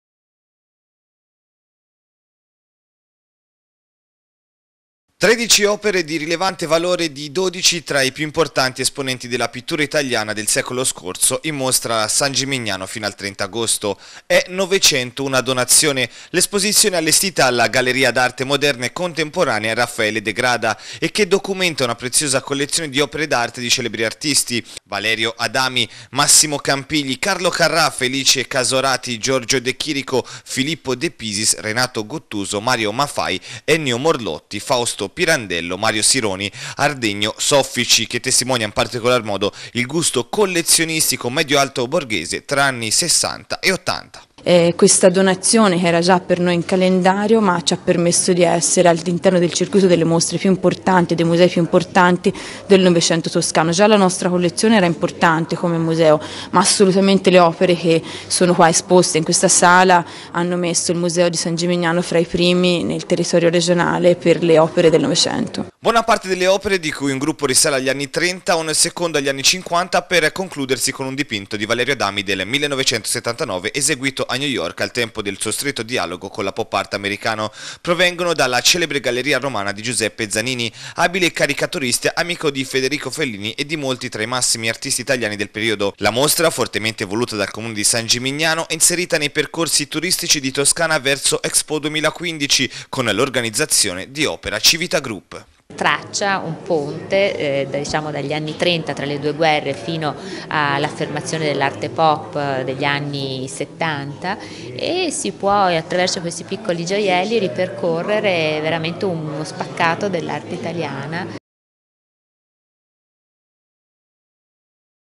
"900. Una donazione" sotto le torri di San Gimignano
Tredici opere di rilevante valore di dodici tra i più importanti esponenti della pittura italiana del secolo scorso in mostra a San Gimignano fino al 30 agosto. E’ “’900. Una donazione” l’esposizione allestita alla Galleria d’Arte Moderna e Contemporanea Raffaele De Grada e che documenta una preziosa collezione di opere d’arte di celebri artisti, Valerio Adami, Massimo Campigli, Carlo Carrà, Felice Casorati, Giorgio De Chirico, Filippo De Pisis, Renato Guttuso, Mario Mafai, Ennio Morlotti, Fausto Pirandello, Mario Sironi, Ardengo Soffici, che testimonia in particolar modo il gusto collezionistico medio-alto borghese fra anni sessanta e ottanta.